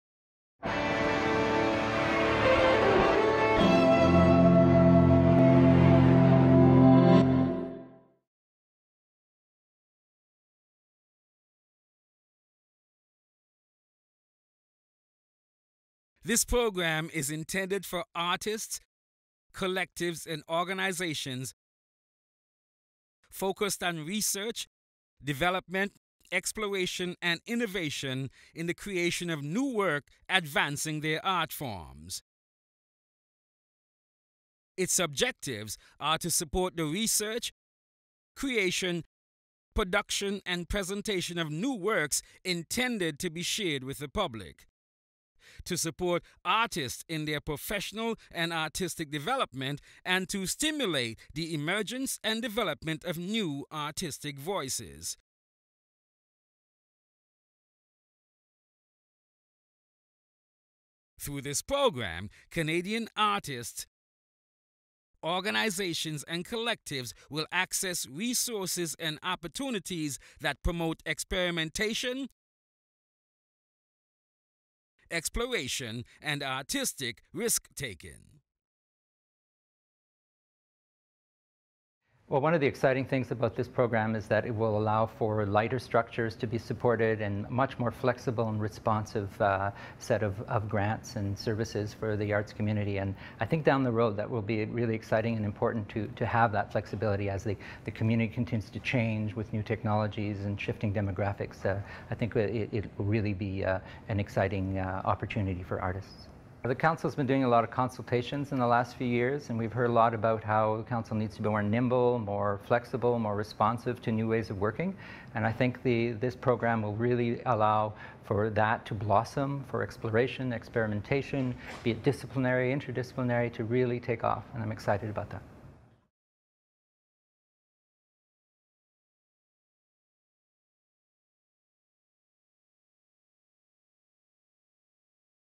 New Canada Council for the Arts Grant Program: Explore and Create (ASL)
The Canada Council for the Arts presents Explore and Create, one of 6 grant programs in our new funding model. In American Sign Language (ASL).